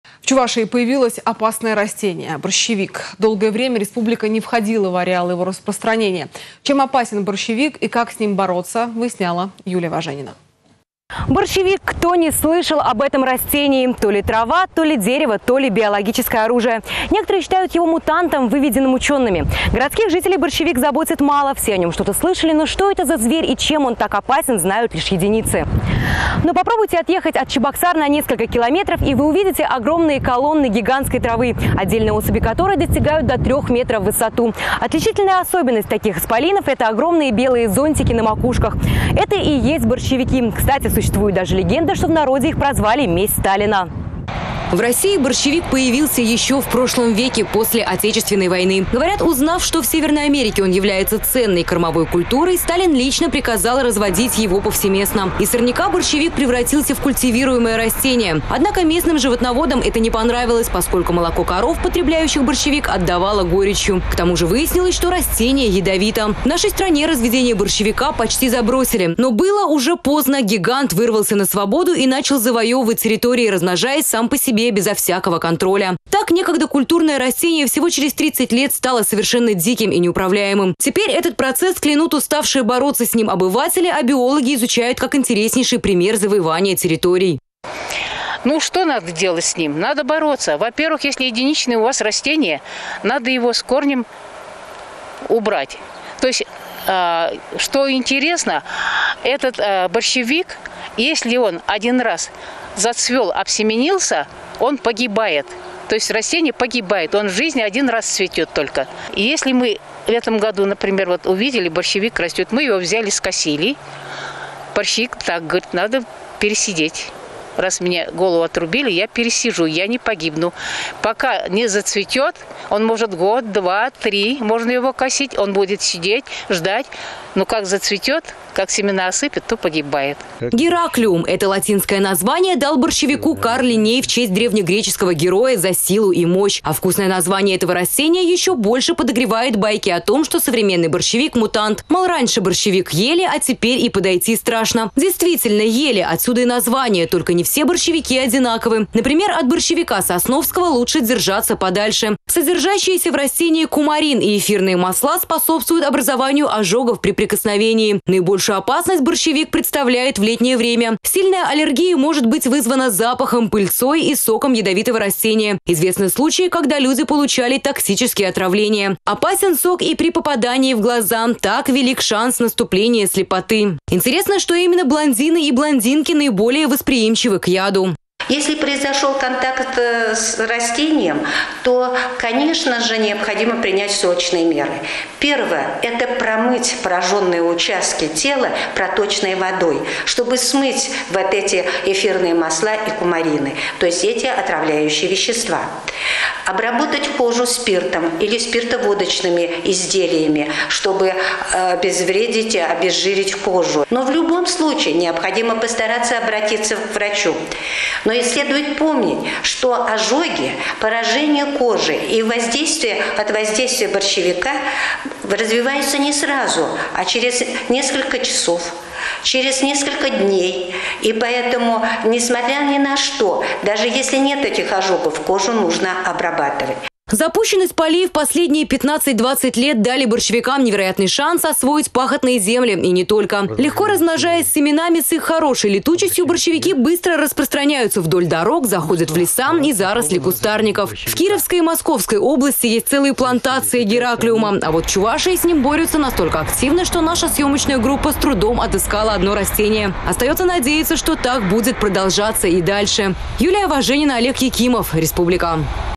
В Чувашии появилось опасное растение — борщевик.
Долгое время республика не входила в ареал его распространения. Чем опасен борщевик и как с ним, бороться выясняла Юлия Важенина.